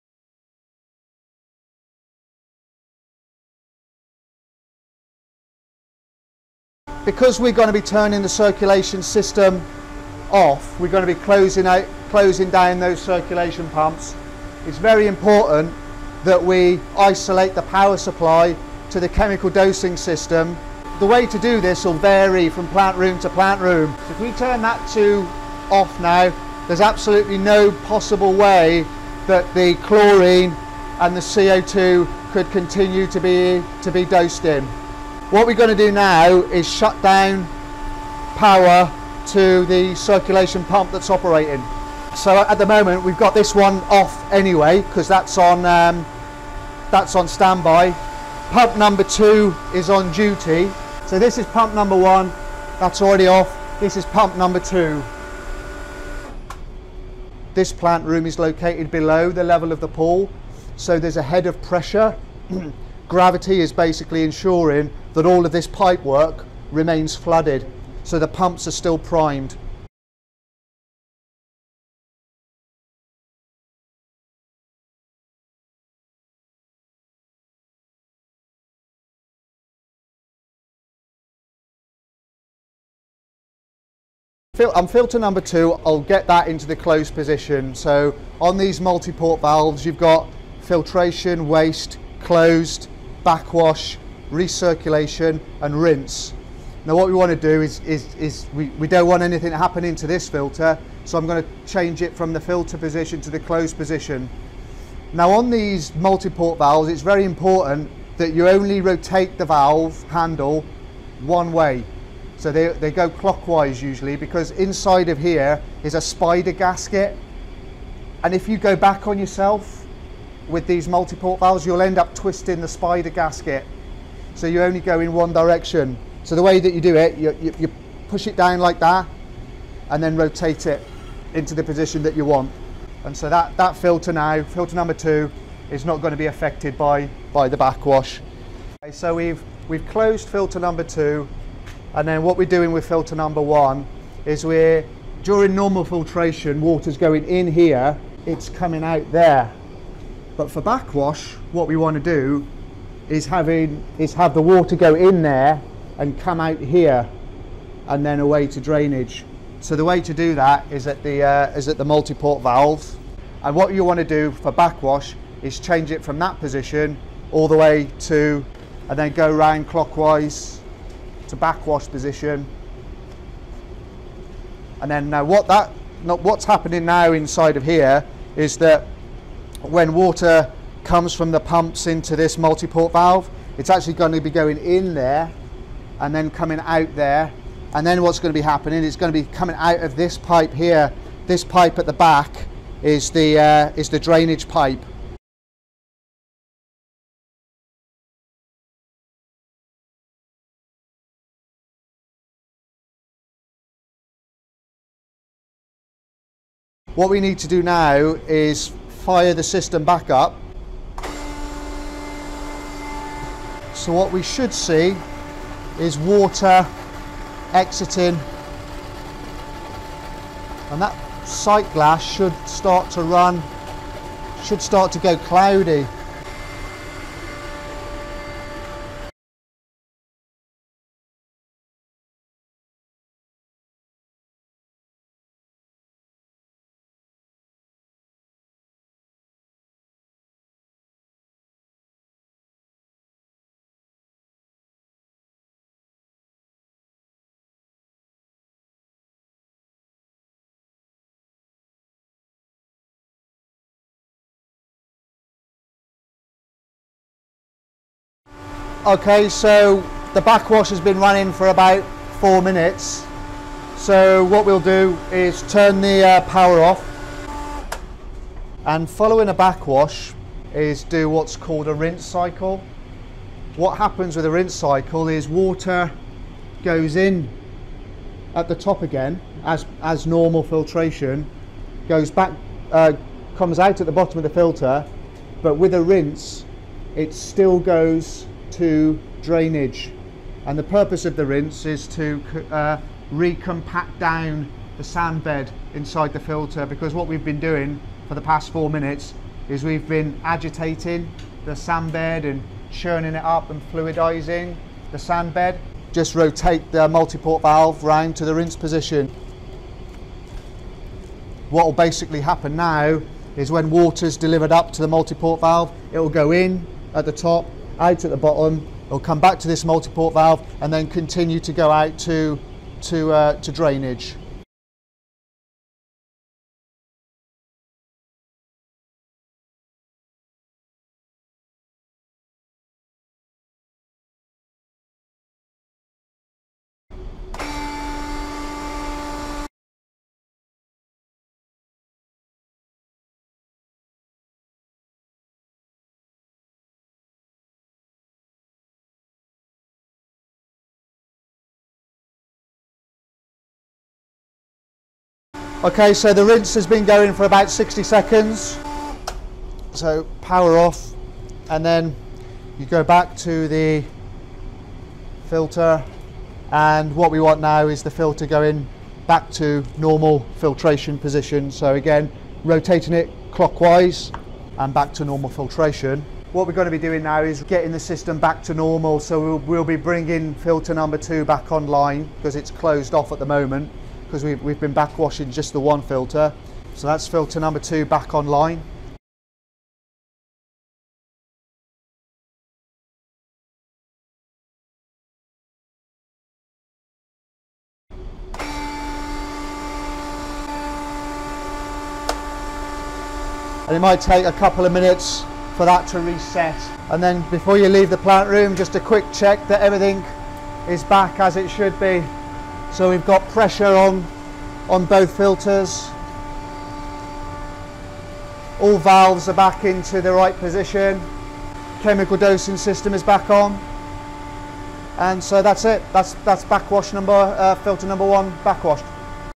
Backwash Demonstration (multiport valve)
In this video, I go through how to backwash a filter with a multiport valve (rather than separate butterfly valves). This system had 2 x filters and 2 x circulation pumps (one on duty and one on standby).

00:00 Turn off circulation and chemical dosing
01:10 Position valve for backwash
03:16 Run backwash
03:51 Position valve for rinse
05:27 Run rinse
05:55 Position valve back to normal filtration
06:37 Circulation and chemical dosing back on